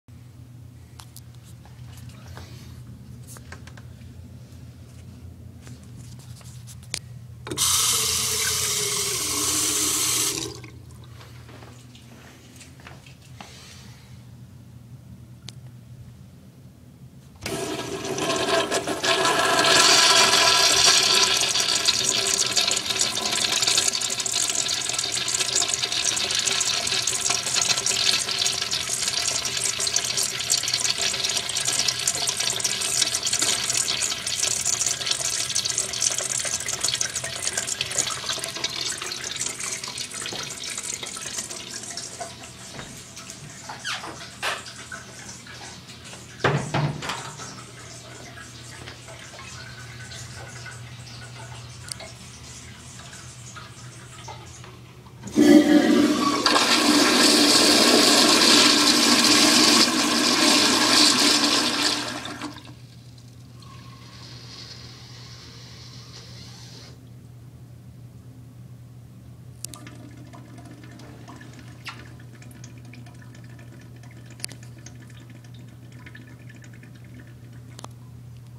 [168] 1996 Kohler Highcliff Toilet/ Dexter Urinal
This is on the bottom floor of one of the dorms on campus, completed in 1996.  Once a coffee shop, it is now the office of residence life.  However, i still got to the back of the office to the old bathroom.  Personally, this is one of the most bizarre toilets i've ever come across, considering the trap size of the dexter urinal is not much smaller than the highcliff; despite that, I still think these are pretty neat.